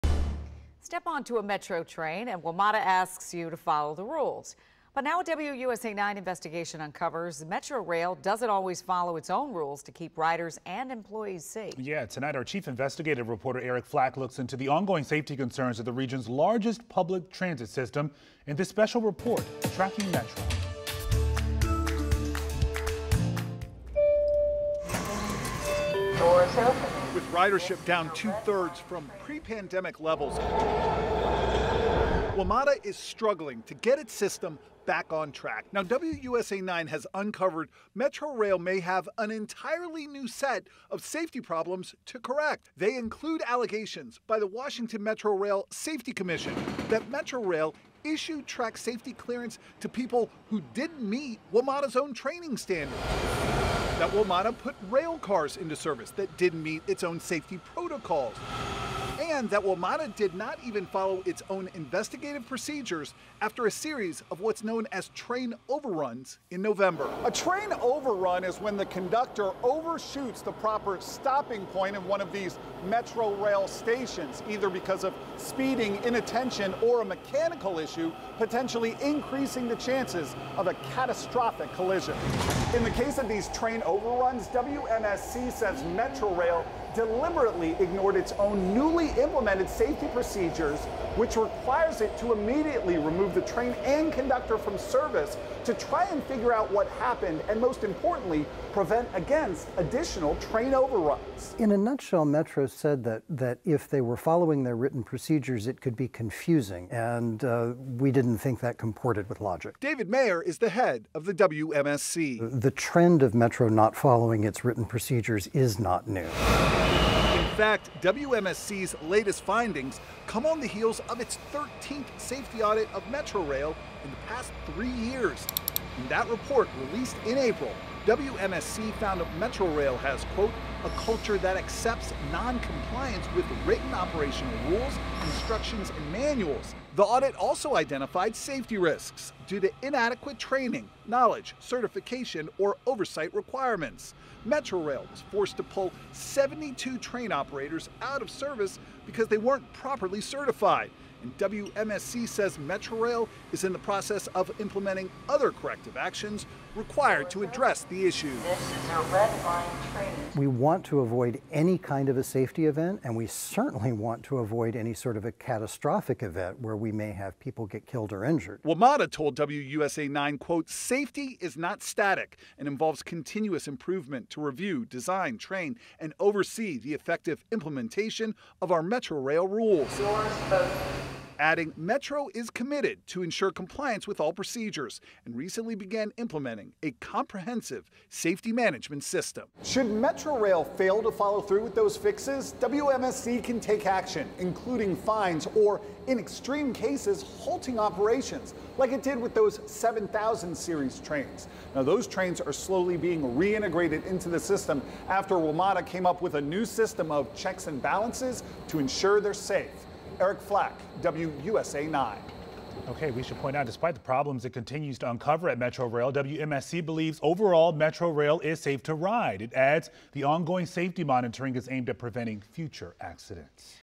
New allegations of safety problems at Metrorail threaten to derail WMATA's overhaul of the system
Fresh accusations include Metrorail didn’t follow WMATA’s own investigative procedures after a series of train overruns in November.